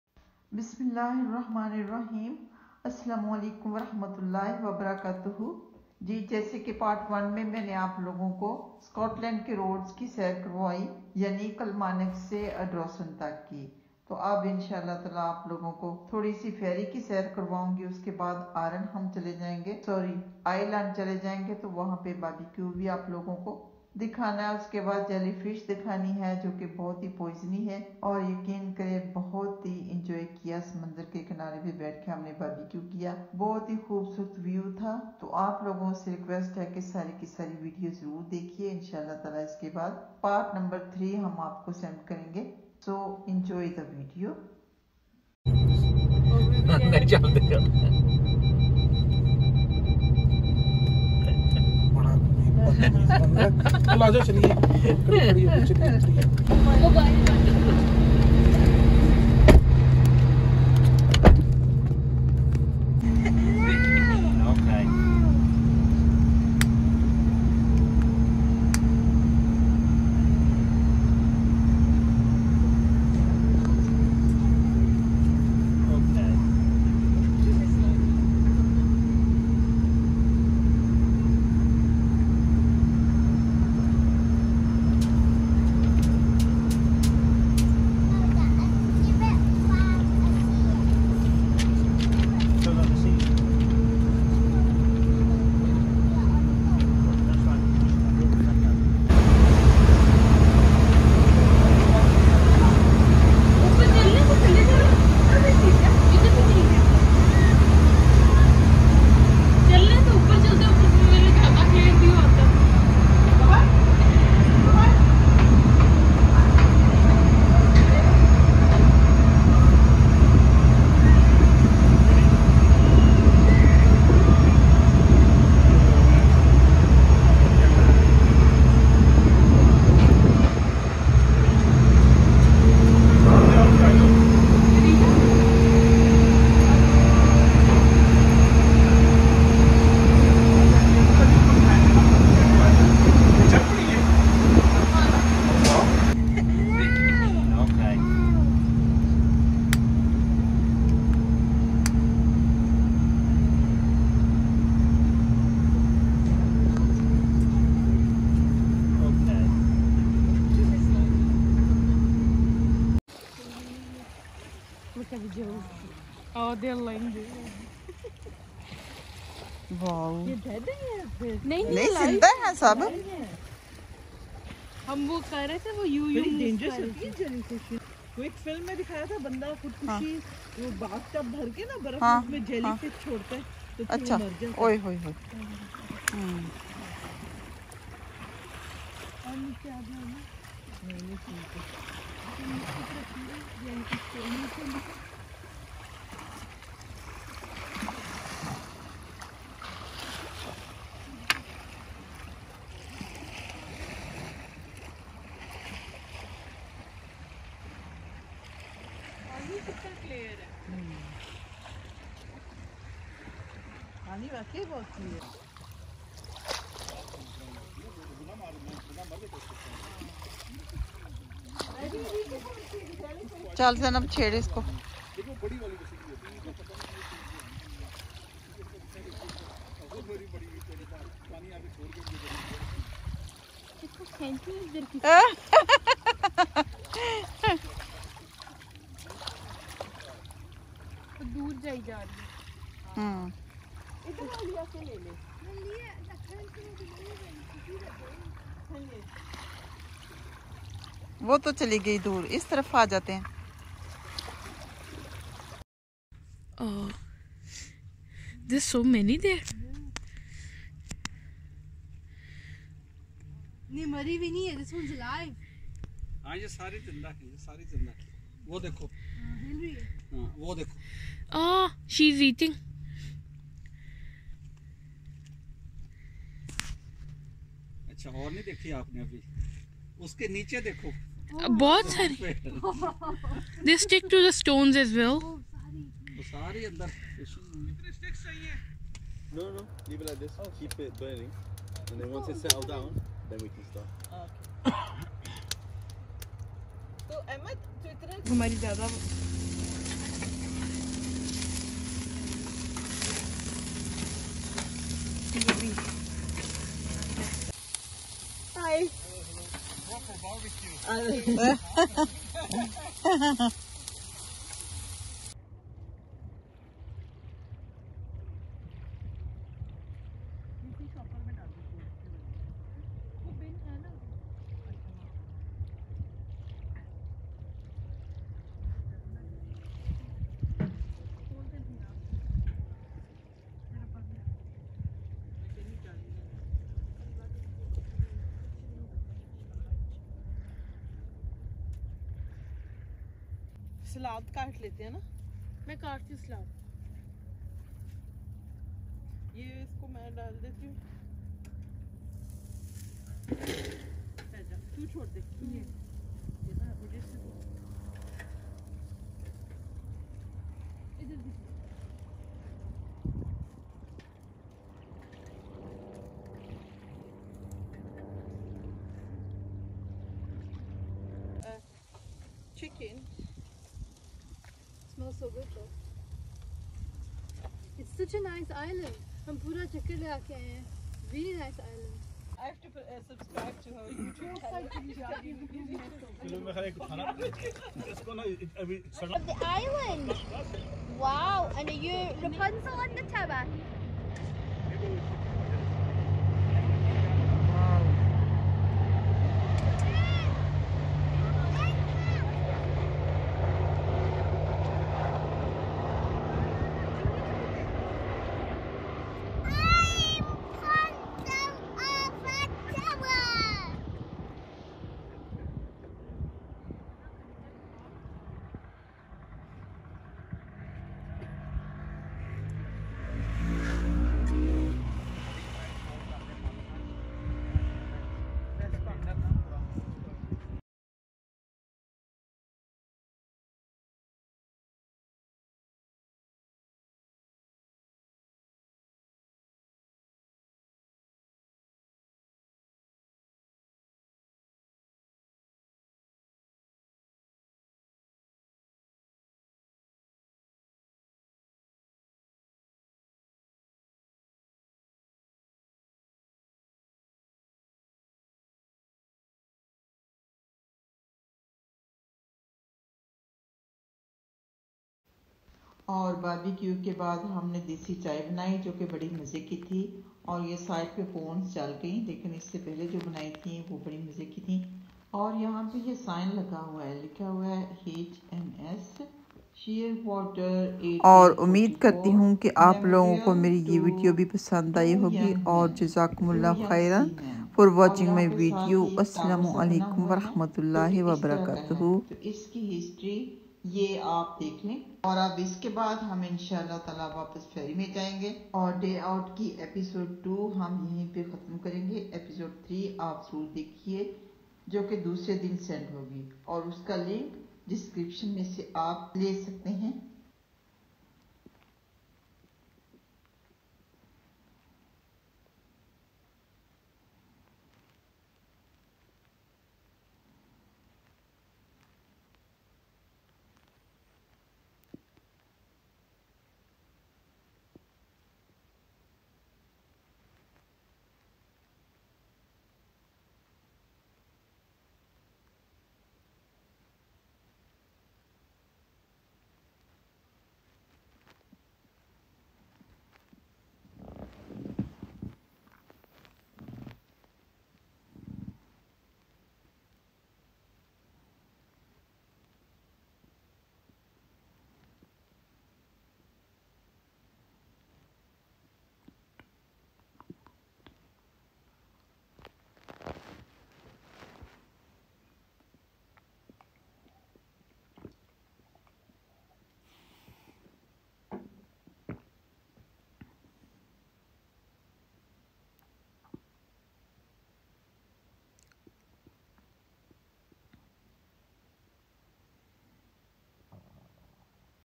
Trip To Island Part 2 |Vlogs|Dunoon Ferry 2022 | Bintehassan
Trip to Islamd part 2, Bismillaah, Enjoy day trip vlog in Island

Places, tourist and travel videos 👇❤️